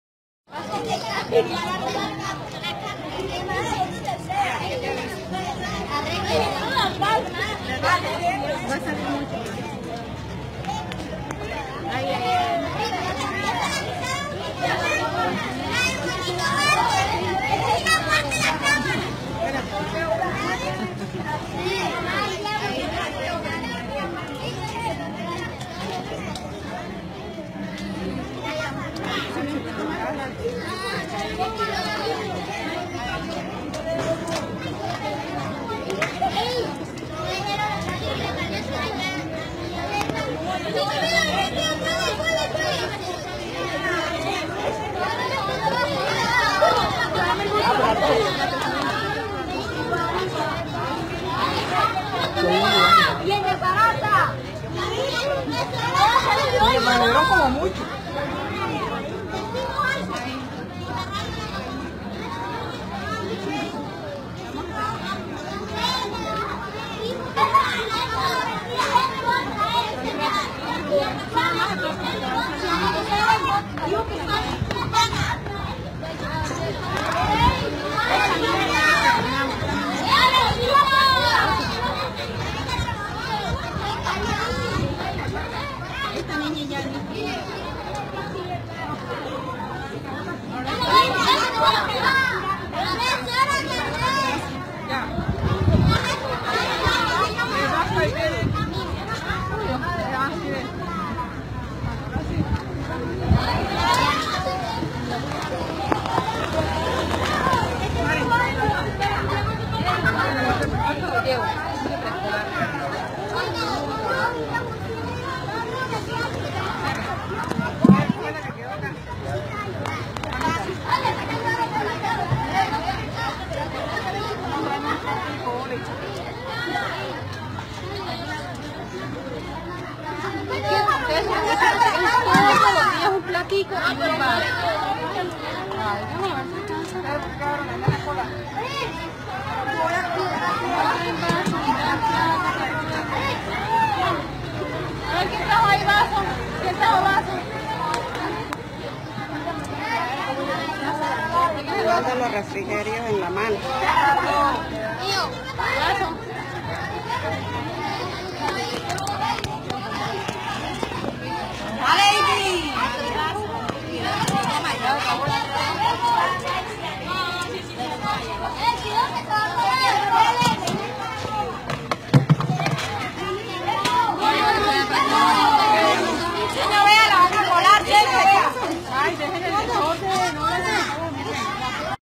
Maestras toman foto engañando que dan de comer bien a niños pero les sirven en la mano.
vídeo Maestras toman foto engañando que dan de comer bien a niños pero les sirven en la mano.

Toman evidencia de los refrigerios usando el mismo plato y en realidad sólo les dan un patacon con tres tiras de pollo, en la mano y sin las más mínimas normas de higiene.
Institución Educativa Sagrado Corazón de Jesús. Aguachica